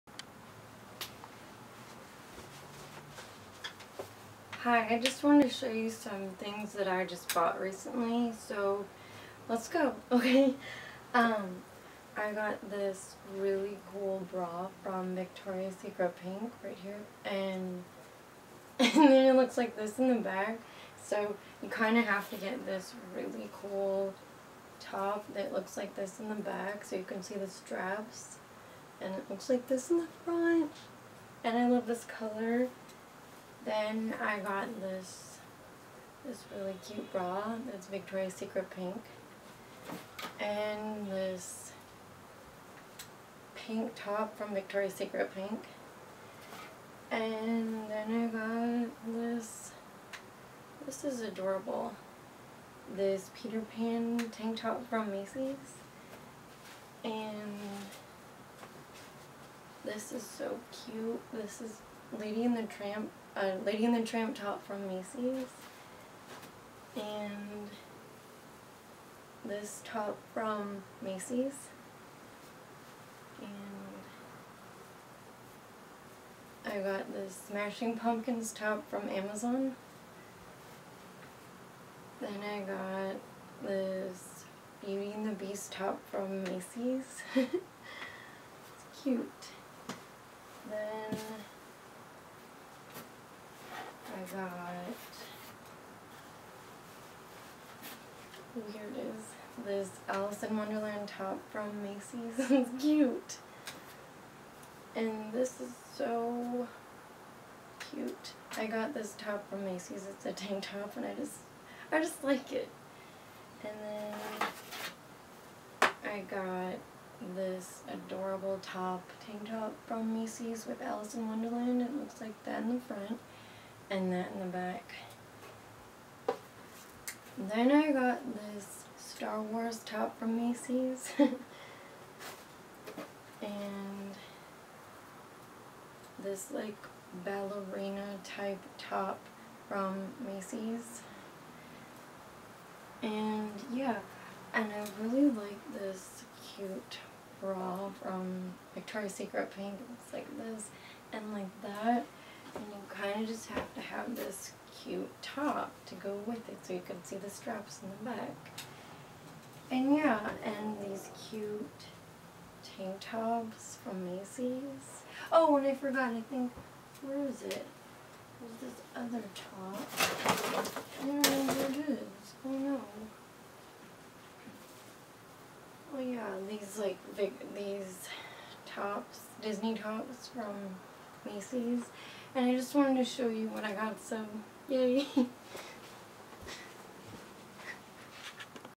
Macy's , Victoria's Secret & Amazon Shopping Haul September 2015
Here are some cute things I bought recently .  Just thought I would share .